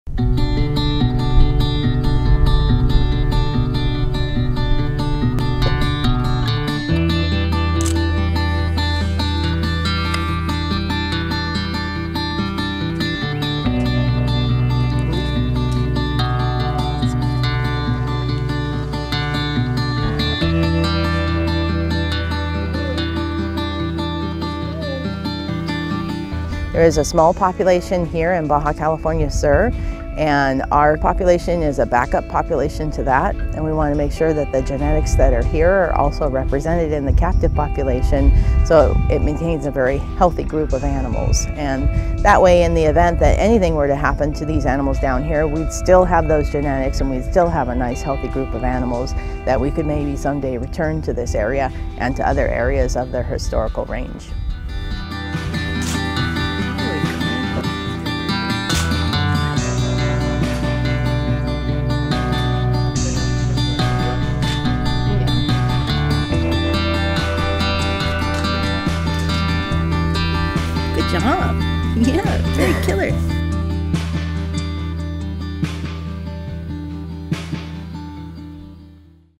Pronghorn | San Diego Union-Tribune
Animal care specialists and veterinarians from five zoos including San Diego Zoo Safari Park and the Los Angeles Zoo are hand raising the baby Pronghorn antelope at El Vizcaíno Biosphere Reserve to sustain the endangered species.